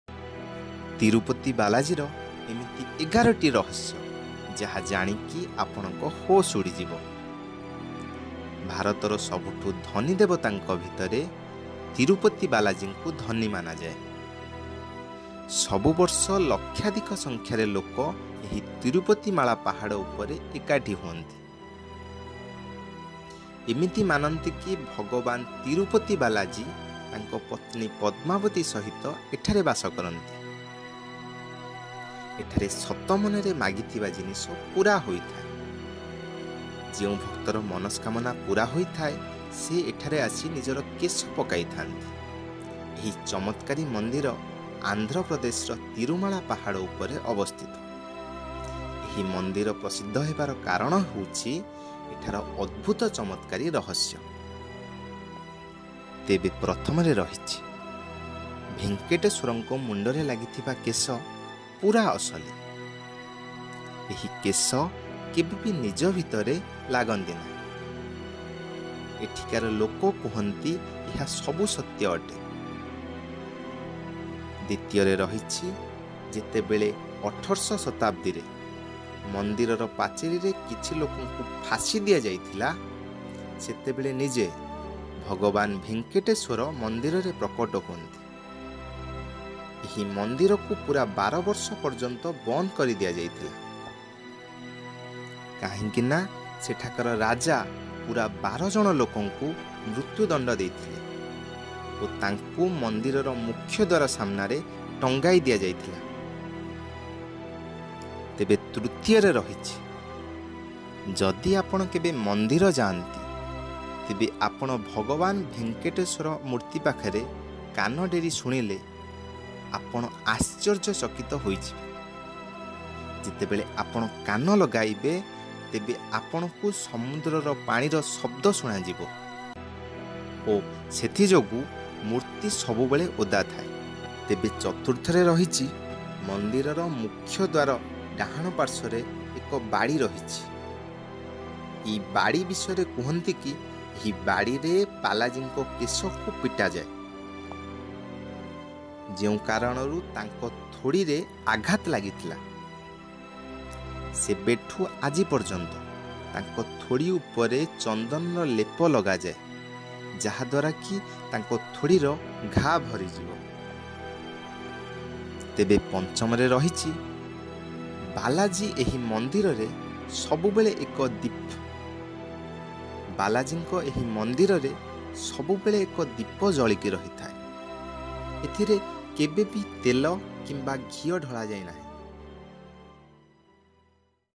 ତିରୁପତି ବାଲାଜୀର ଏମିତି ୧୧ଟି ରହସ୍ୟମୟ କଥା ଯାହା ଜାଣିଲେ ଆପଣଙ୍କ ହୋସ୍ ଉଡ଼ିଯିବ
Follow Us on - ( Facebook - Instagram - Twiter )